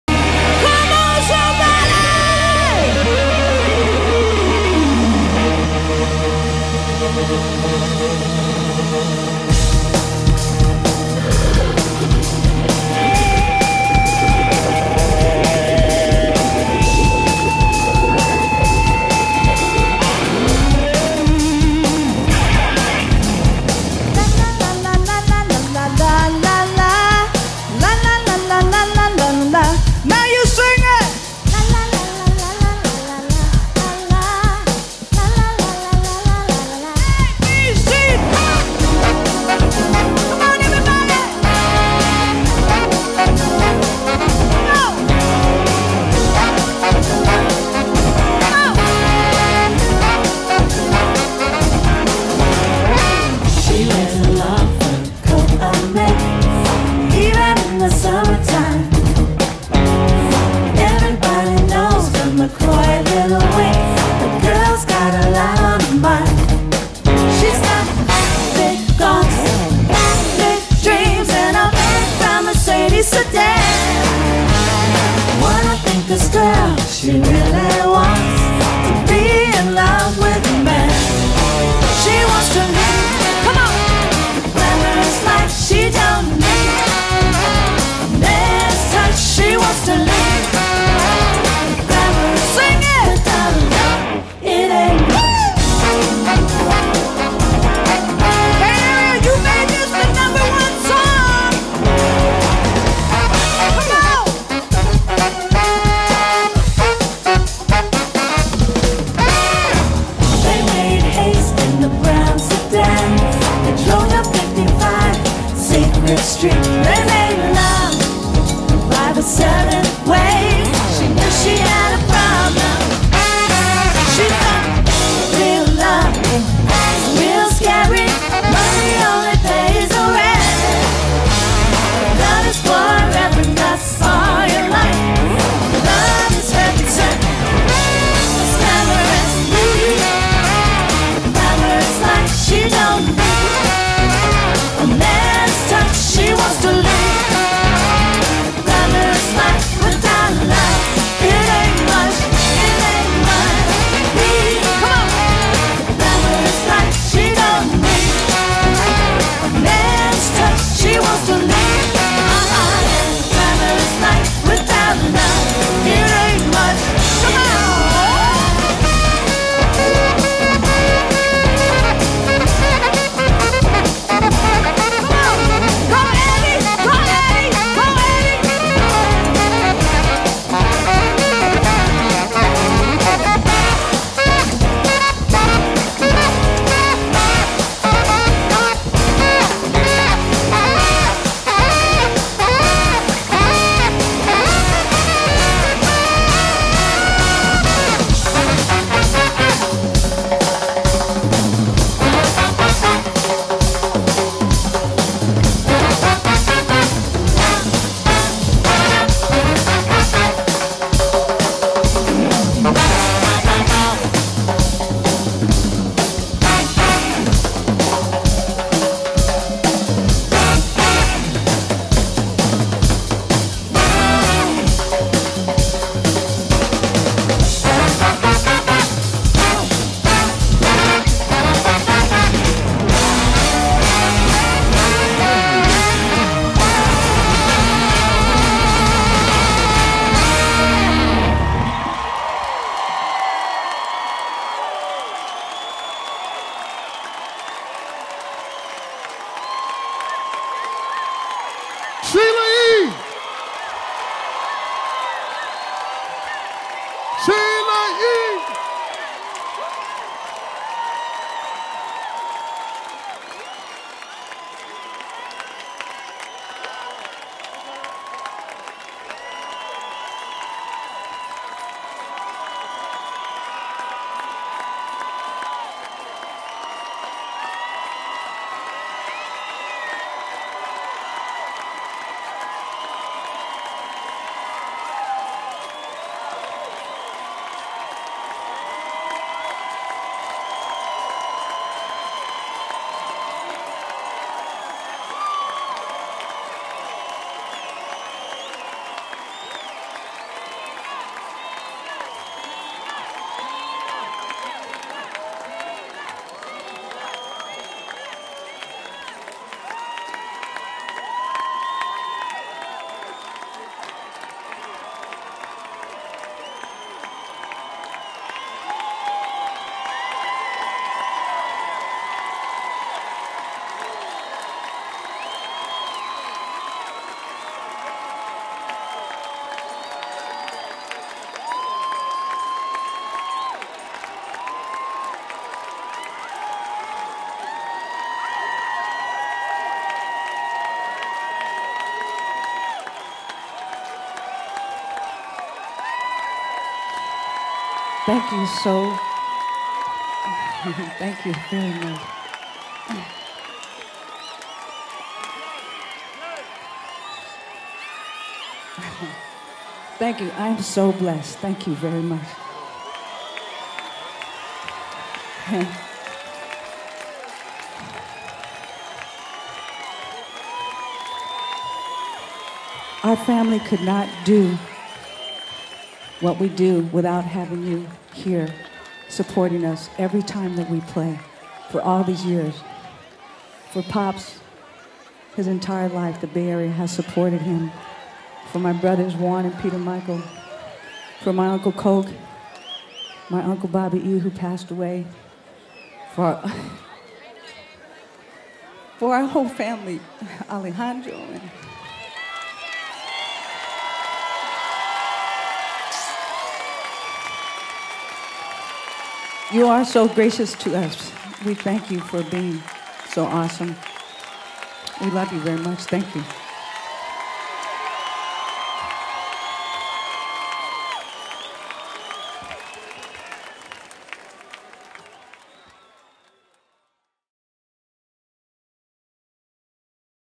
Sheila E Live at Stern Grove Festival
Sheila E performing live at Stern Grove Festival on July 22, 2012.